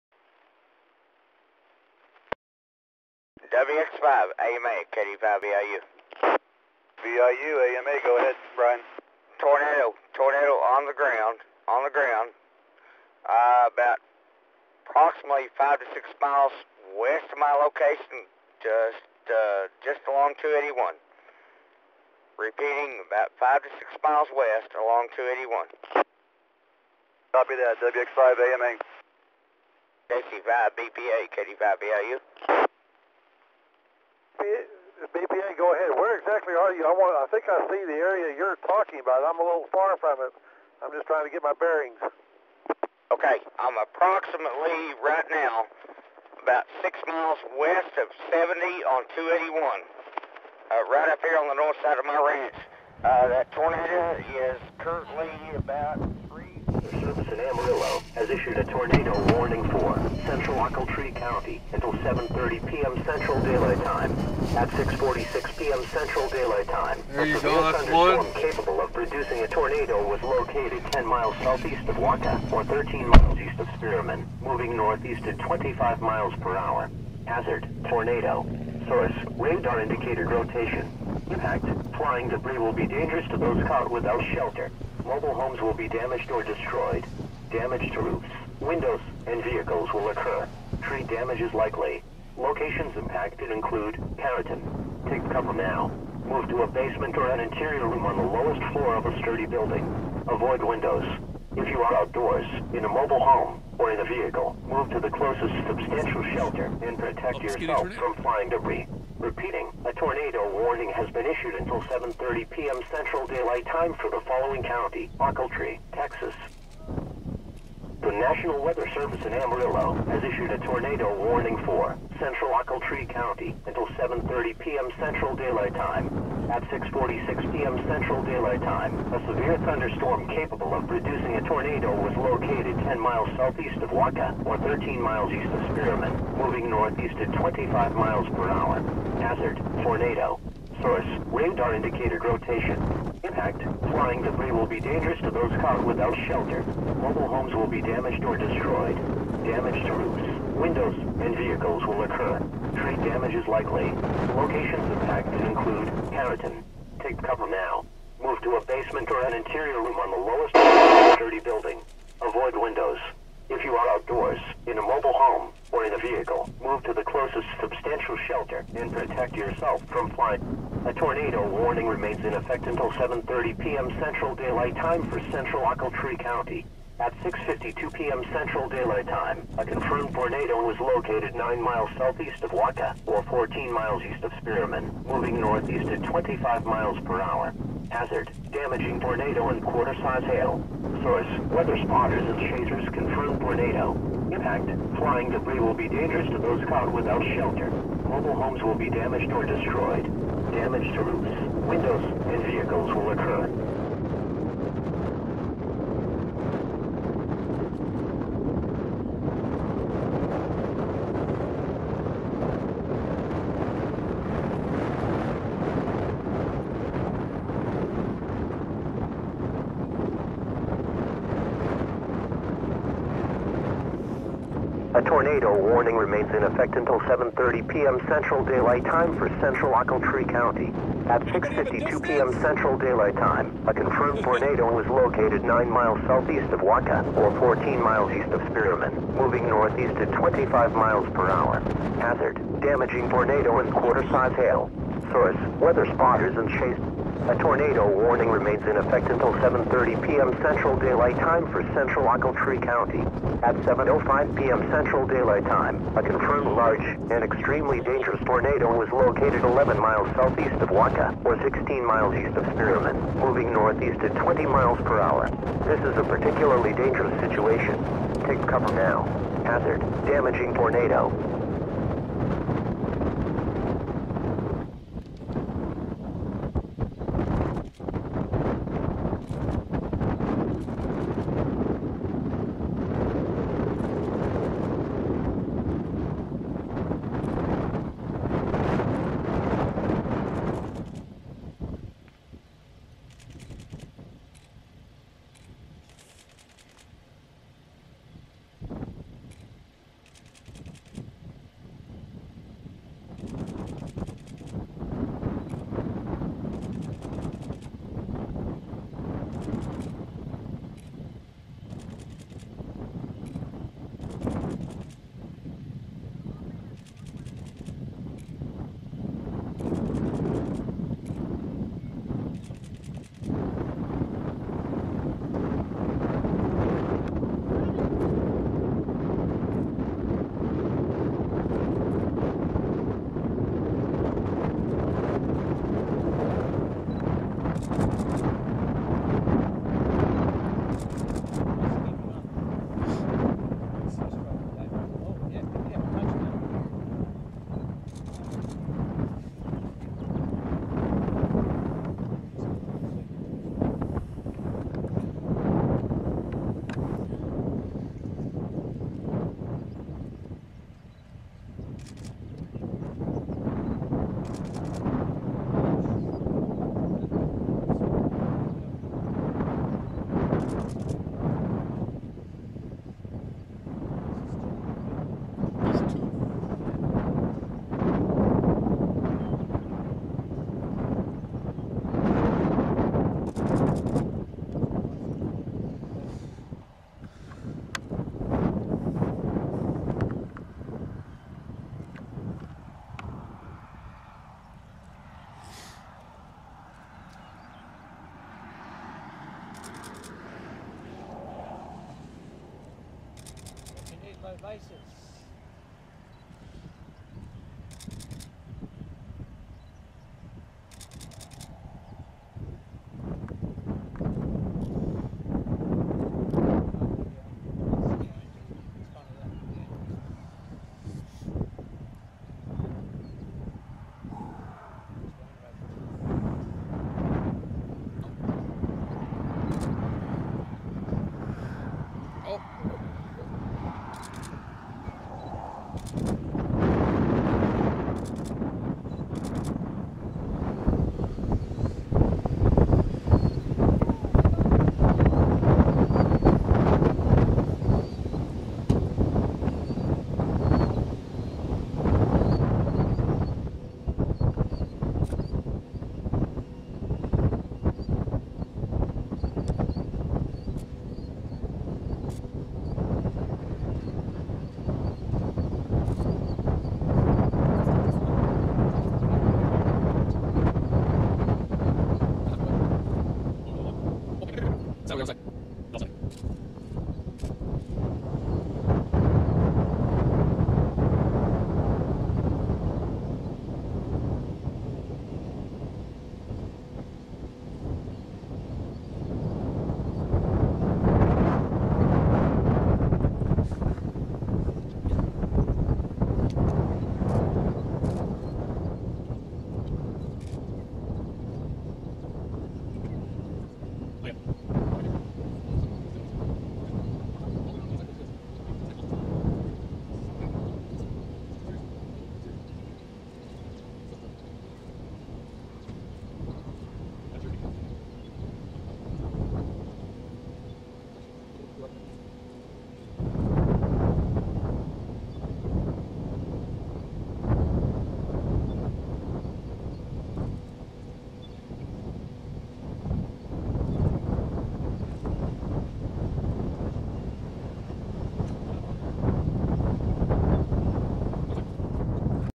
Multiple Tornadoes near Perryton Texas - May 22nd 2016
Multiple tornadoes descend from a supercell thunderstorm in Ochiltree County 7 miles southwest of Perryton, Texas on the evening of May 22nd 2016.